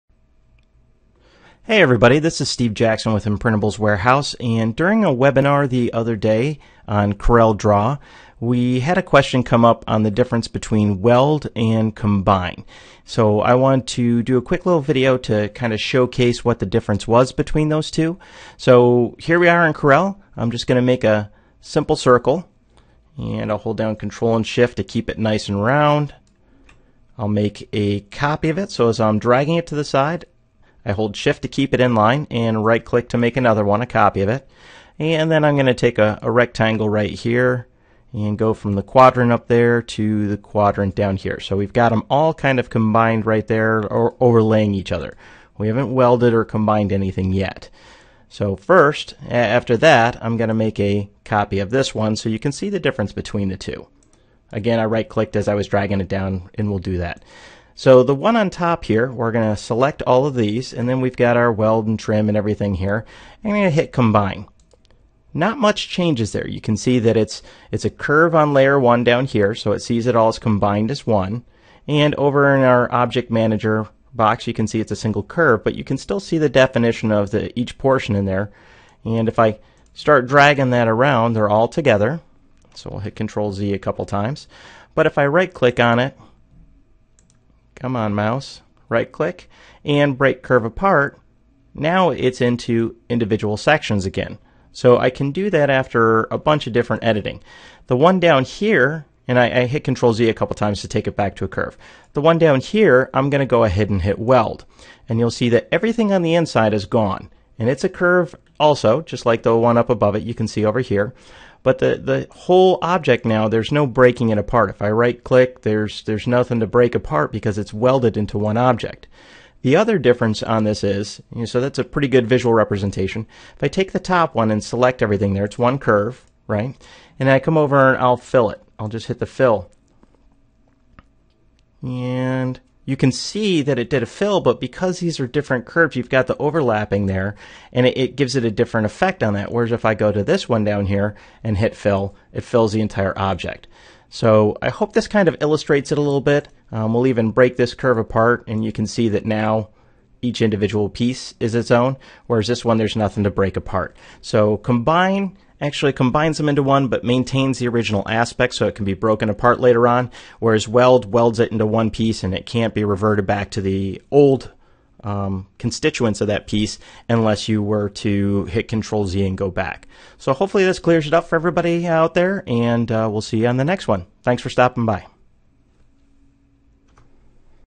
Corel Draw Combine vs Weld
From a webinar on Corel Draw the other day there was a question on what the difference between Weld and Combine were. This video shows what each does and explains the differences.

As always, I hope this helps and let me know what questions you have!

~Steven